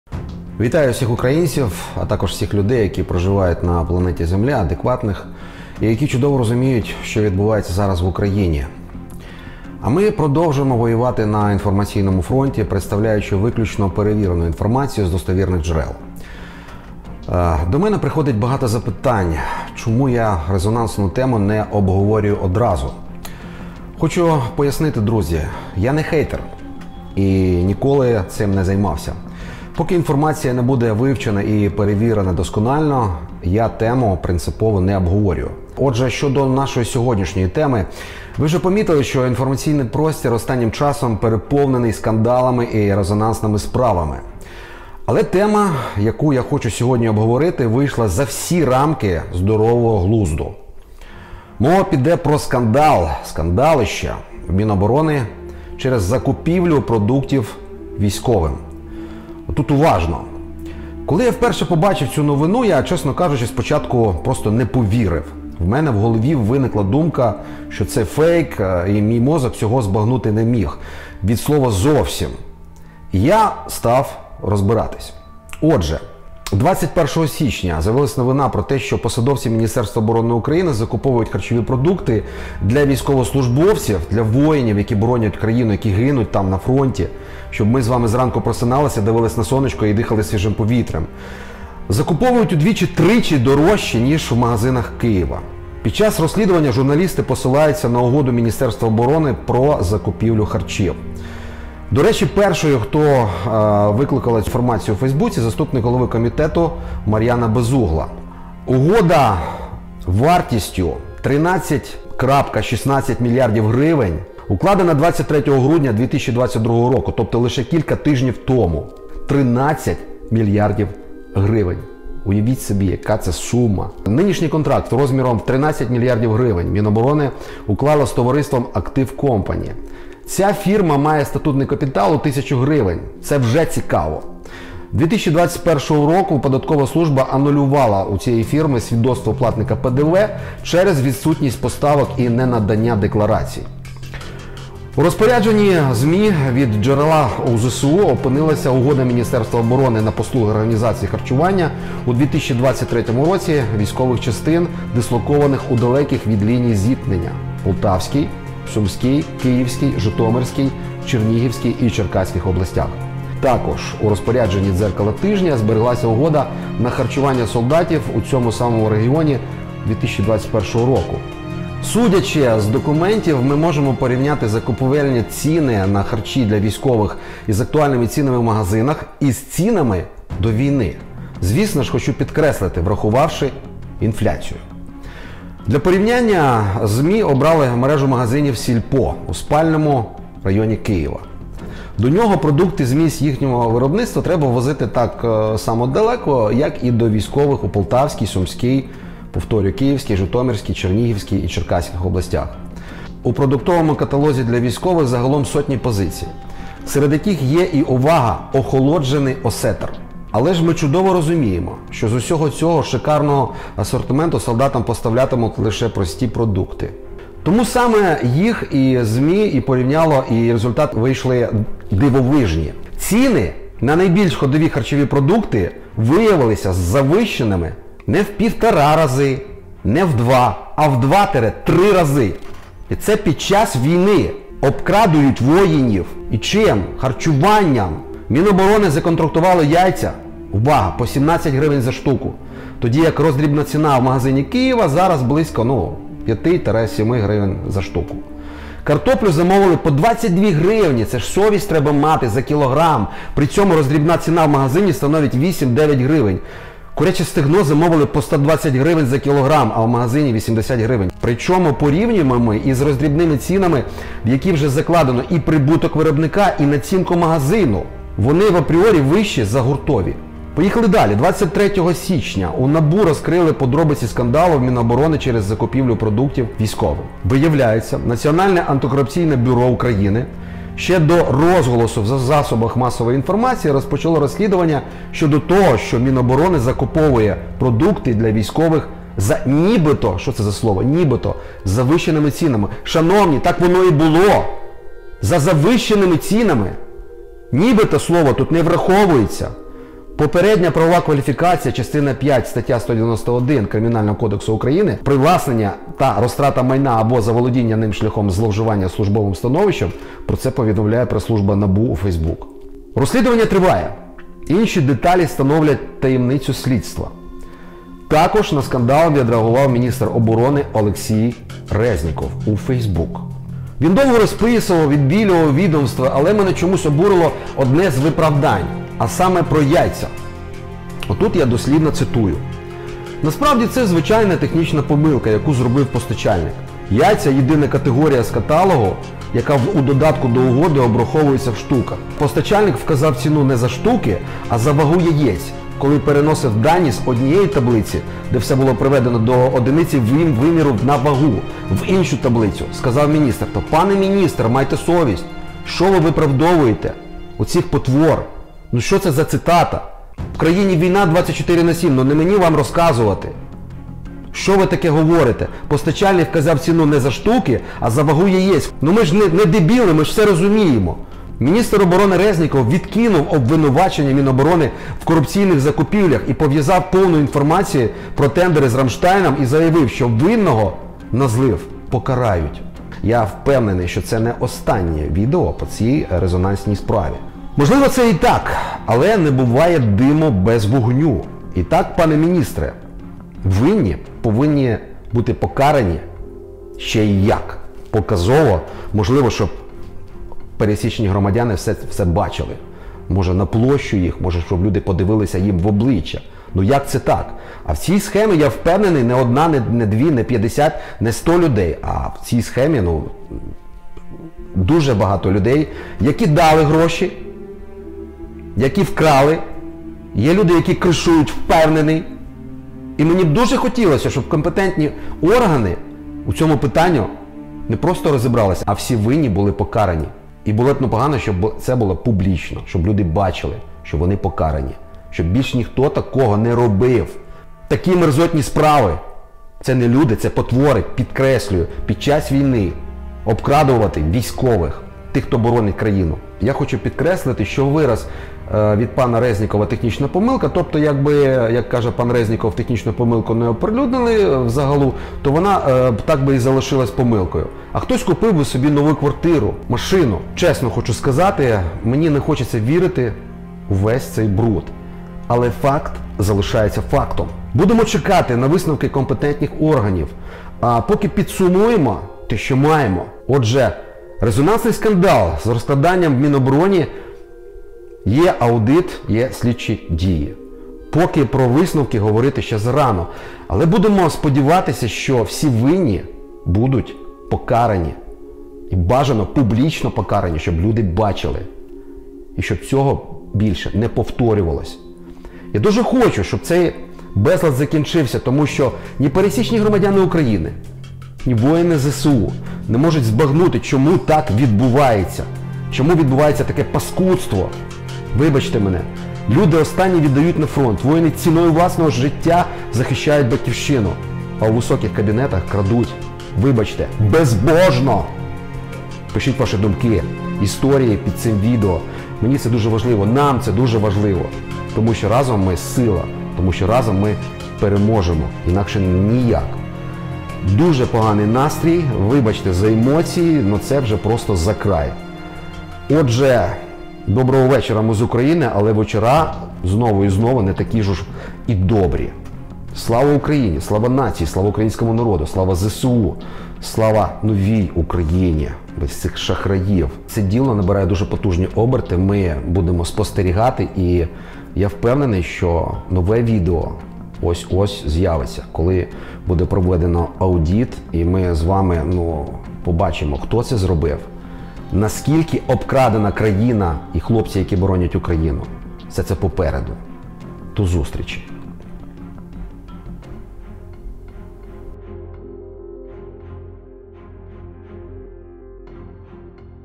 Обкрадання воЇнів ЗСУ! Тилові пацюки в Мін Оборони України!!
Отже що до нашої сьогоднішньої теми. Ви вже помітили що інформ. простір останнім часом переповнений скандалами і резонансними справами. Але тема яку я хочу сьгодні обговорити, вийшла за всі рамки здорового глузду. Мова піде про скандал в Міноборони через закупівлю продуктів військовим.
Коли я в перше побачив цю новину, я чесно кажучи спочатку не повірив. В мене в голові виникла думка що це фейк, мій мозок цього збагнути не міг, від слова зовсім.І я став розбиратися.
Отже.
21 першого січня з’явилась новина про те, що посадовці Міністерства оборони України закуповують харчові продукти для військовослужбовців у двічі-тричі дорожче, ніж у магазинах Києва. Під час розслідування журналісти посилаються на угоду Міністерства оборони про закупівлю харчів.